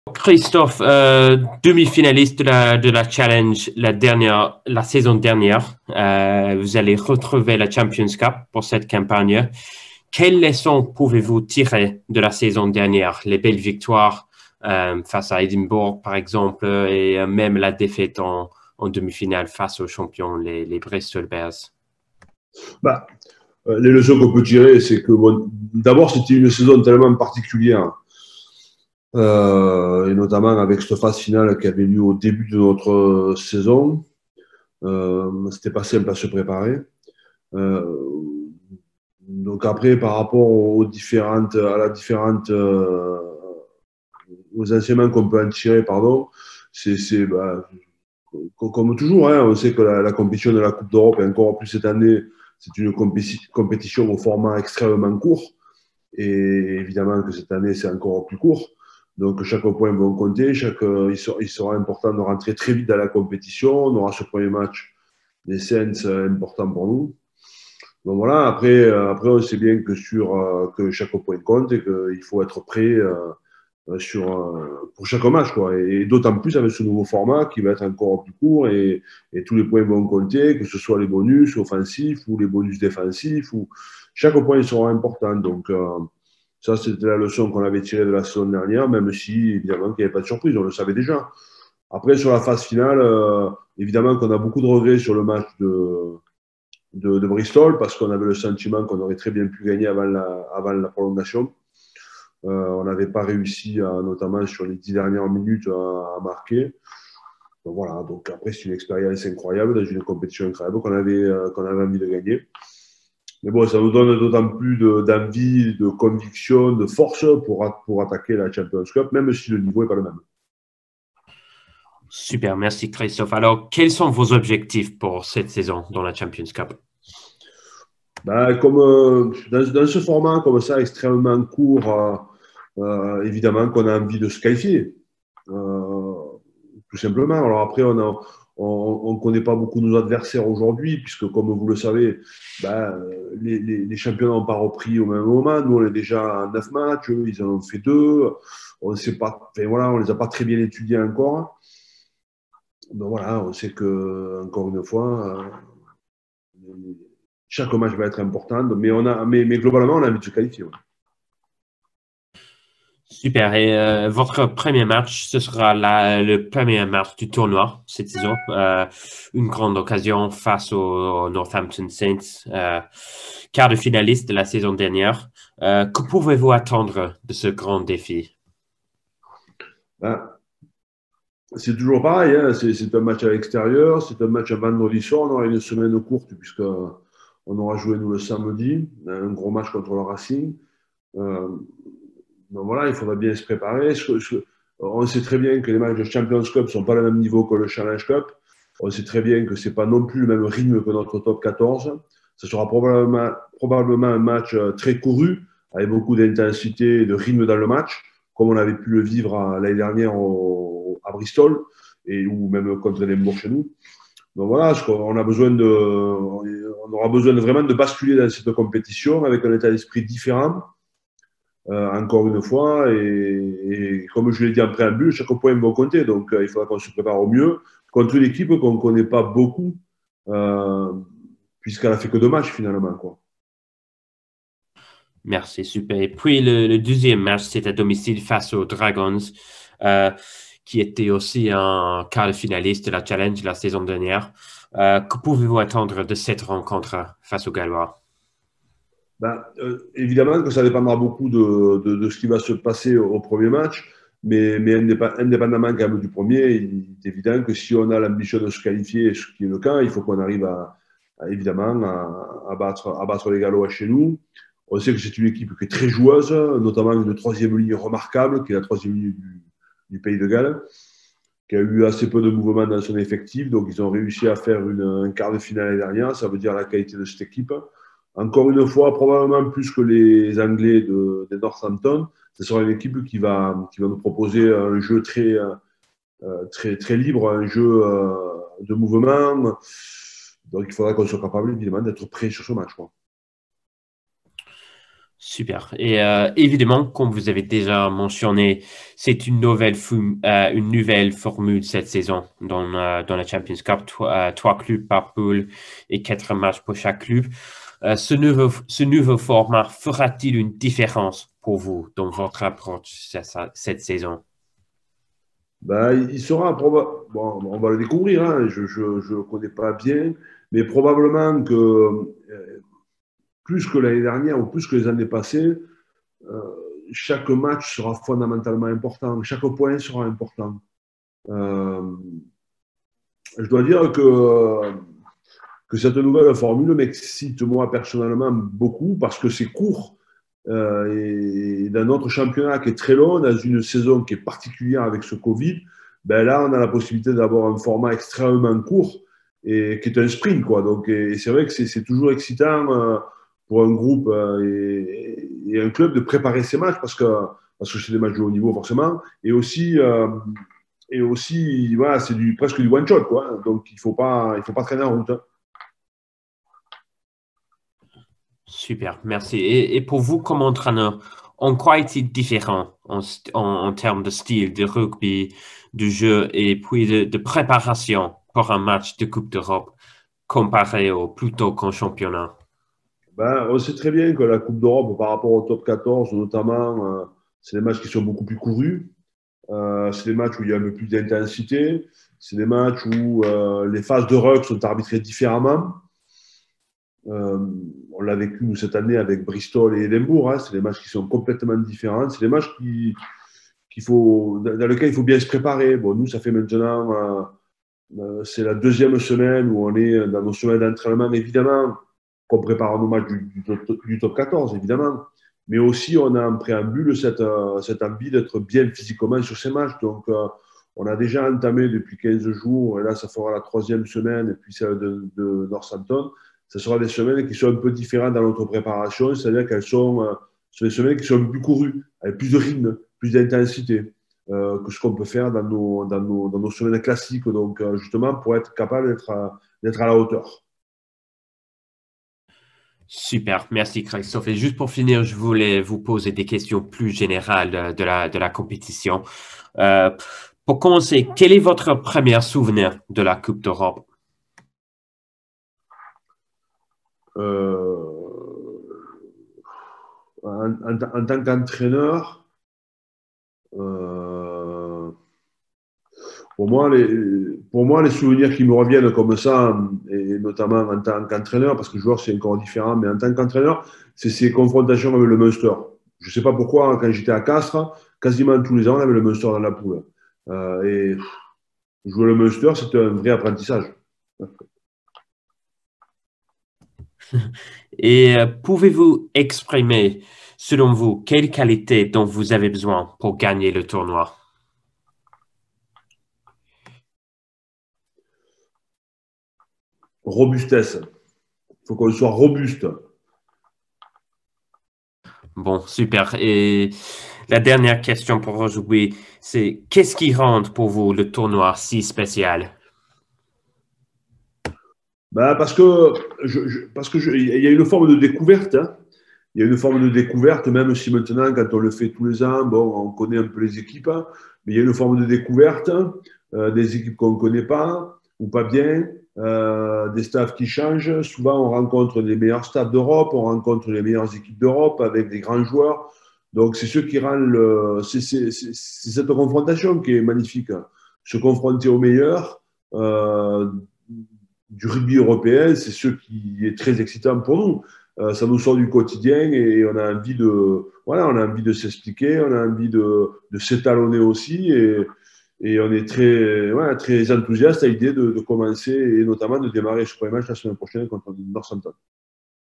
Bordeaux : revivez la conférence de presse de Christophe Urios
Avant cette première journée de Champions Cup, Midi Olympique vous fait revivre les conférences de presse des entraîneurs et joueurs des clubs français en compétition.